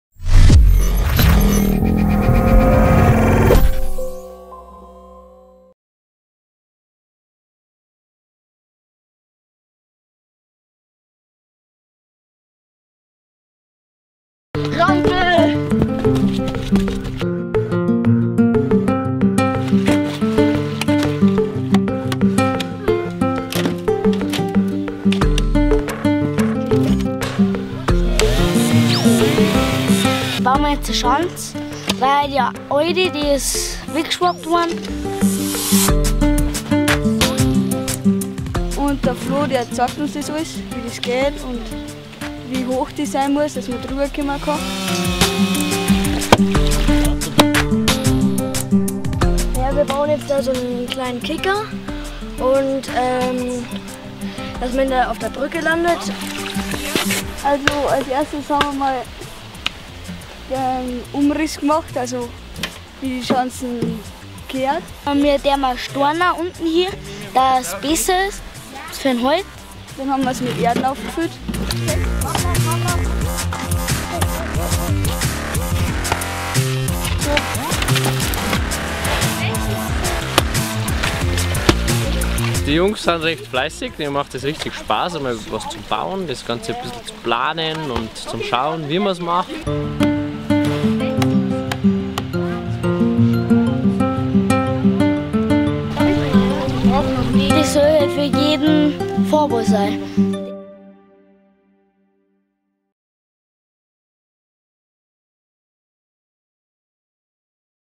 ONE AND A HALF MEN
Kids haben Spaß, beim TEVA Mountainbike Camp mit Flo Gottschlich am Spielberghaus in Saalbach-Hinterglemm.